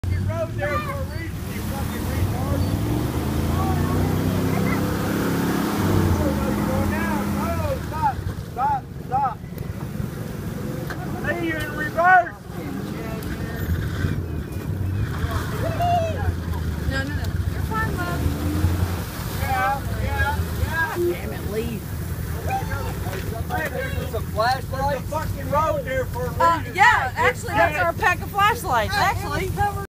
Pathfinder does it again
Pathfinder pulling a truck out